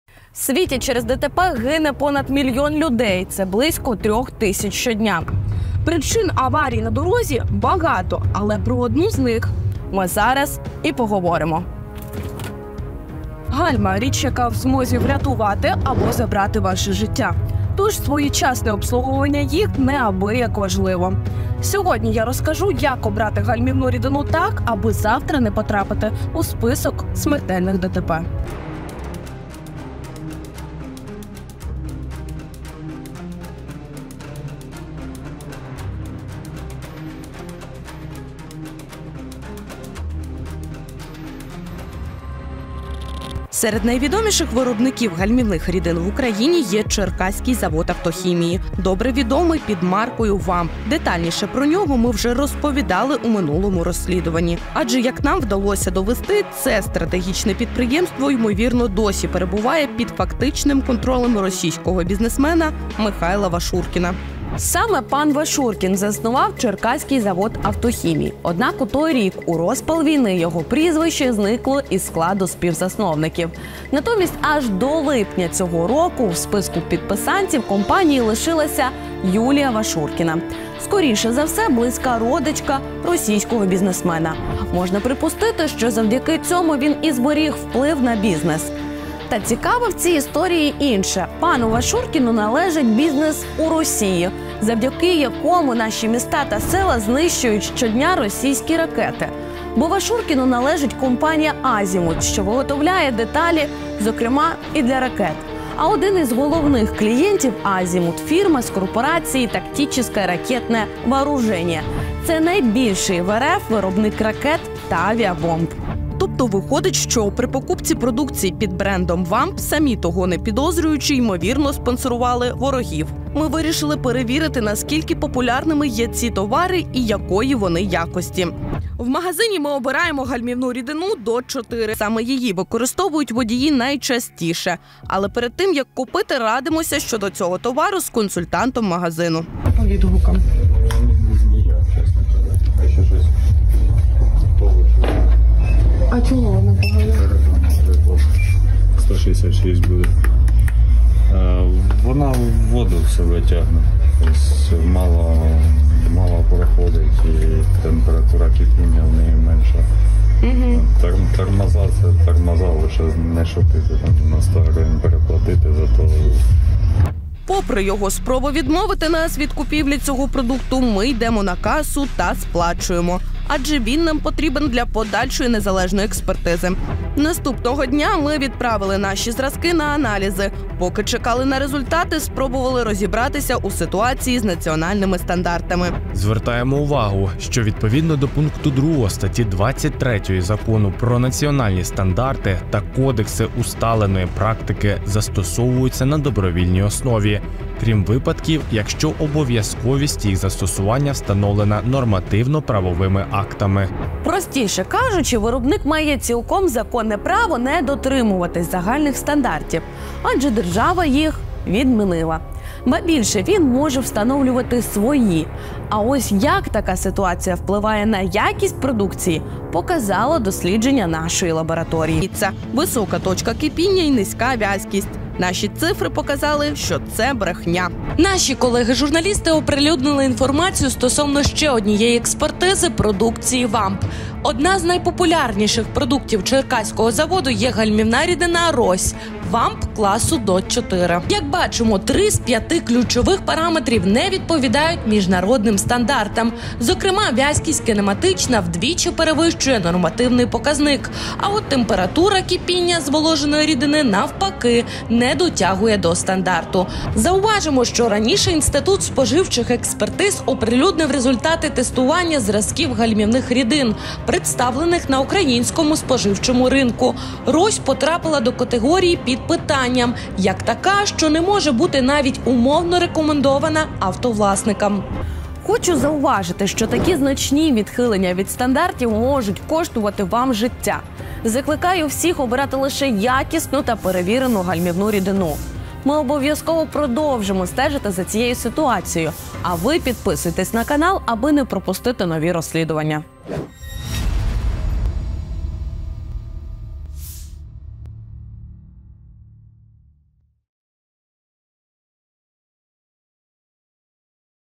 Нові ДТП через відмову гальм? Яку рідину краще не заливати в автомобіль? | СтопКор
У світі внаслідок ДТП гине понад мільйон людей за рік, або в середньому близько трьох тисяч щодня. Причин аварій на дорозі багато, і про одну з них ми зараз і поговоримо. Гальма - річ, яка в змозі врятувати або забрати ваше життя. Тож своєчасне їх обслуговування неабияк важливе! Сьогодні я розкажу, як обрати гальмівну рідину так, аби завтра не потрапити у статистику смертельних ДТП.